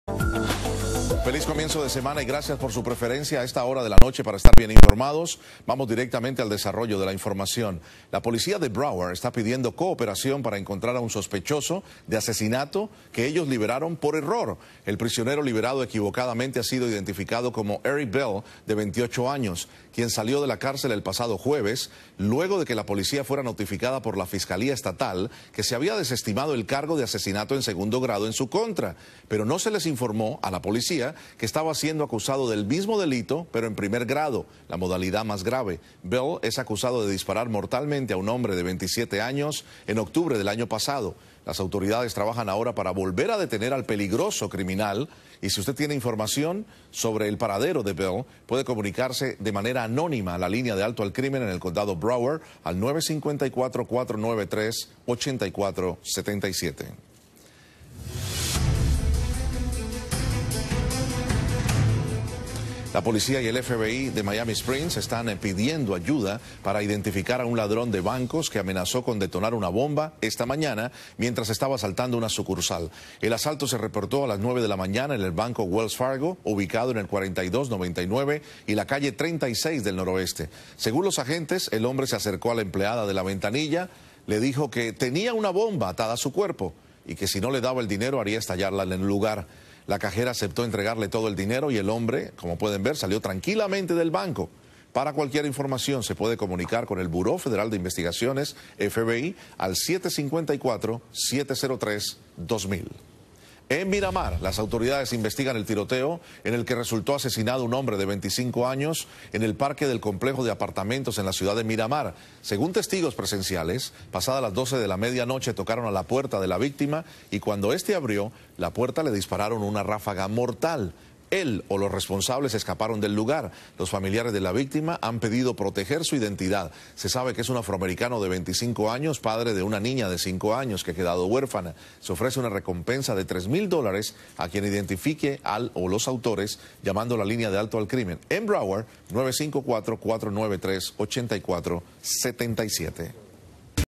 Roban un banco en Miami Springs, Florida y amenazan con hacer explotar una "Bomba"
En el Sur de la Florida 06-03-19 roban un banco en Miami Springs y amenazan con hacer explotar una "Bomba"